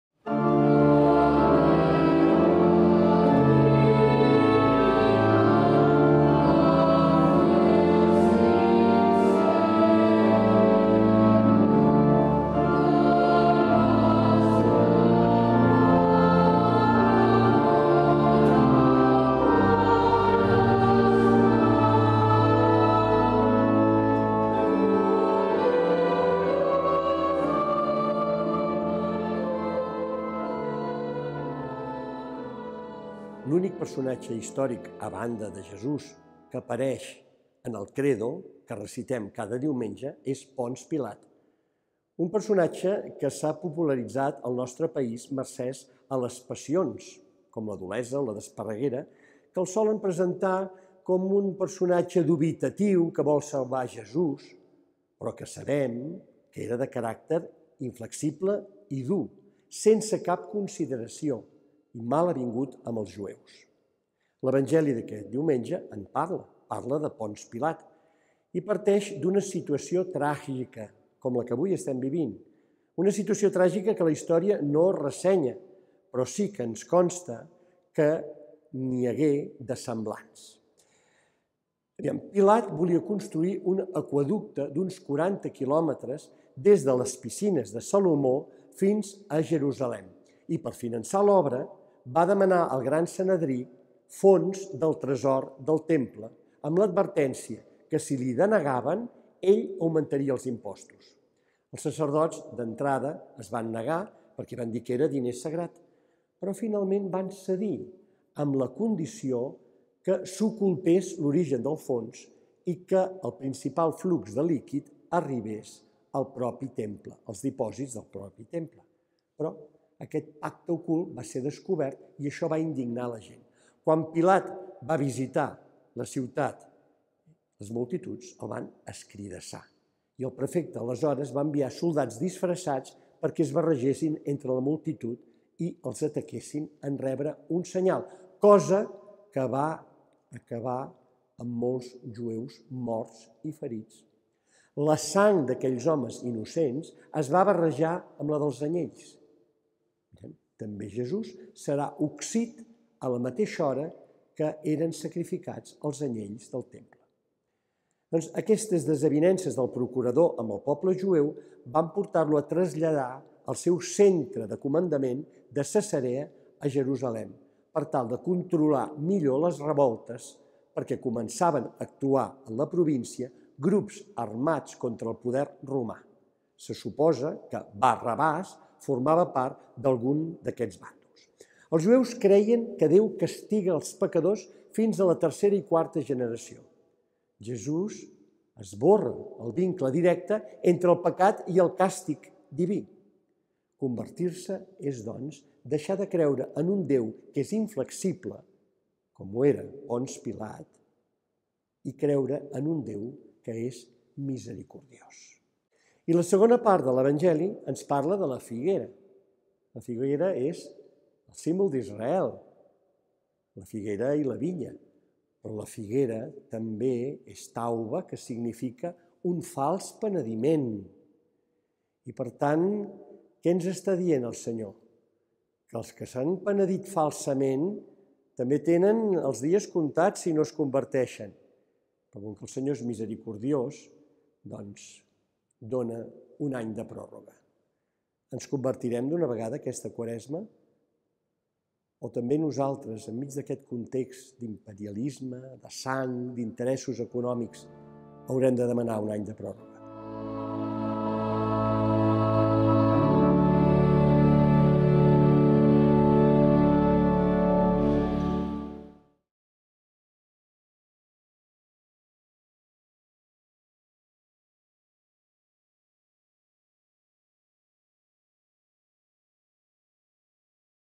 Comentari del Diumenge III de Quaresma (2022)
Jaume Aymar, rector de la parròquia de Santa Maria de Badalona, comenta el tercer diumenge d'aquest temps litúrgic.